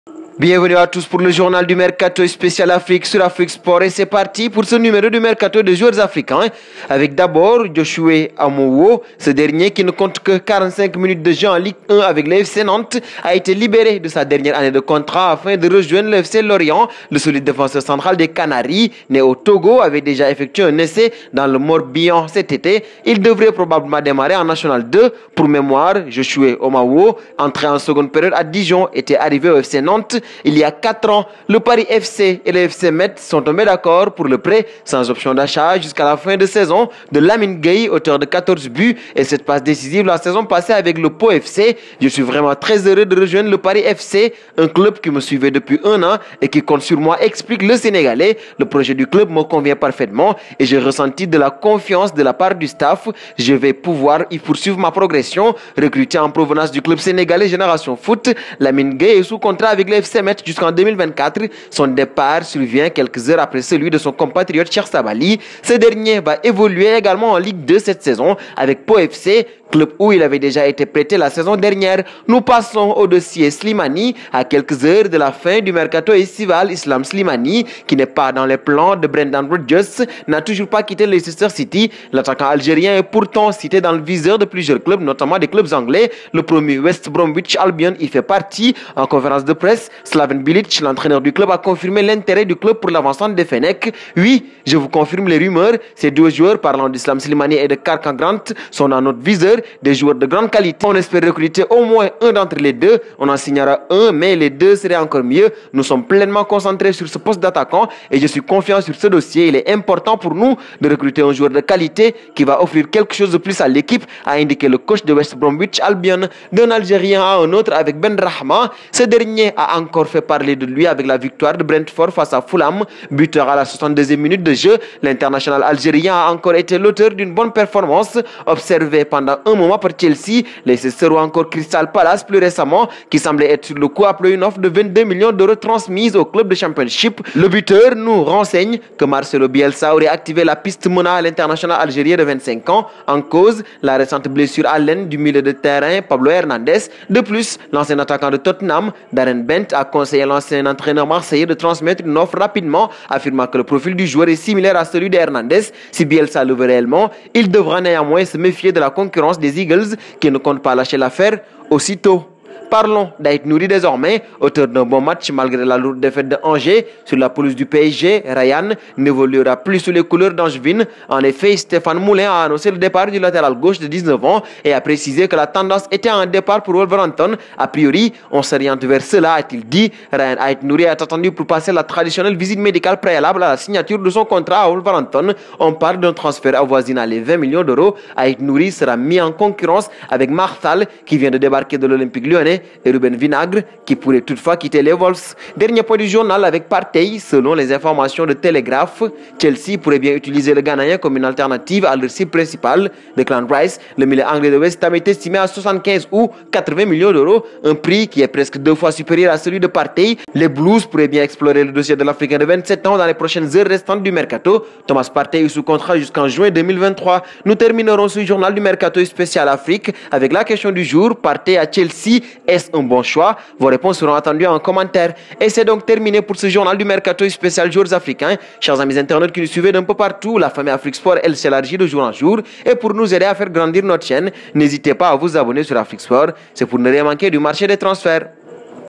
Chelsea veut signer une autre star africaine... Islam Slimani intéresse en Angleterre | Mercato
Question du jour : Partey à Chelsea, un bon choix ? Vos réponses seront attendues en commentaires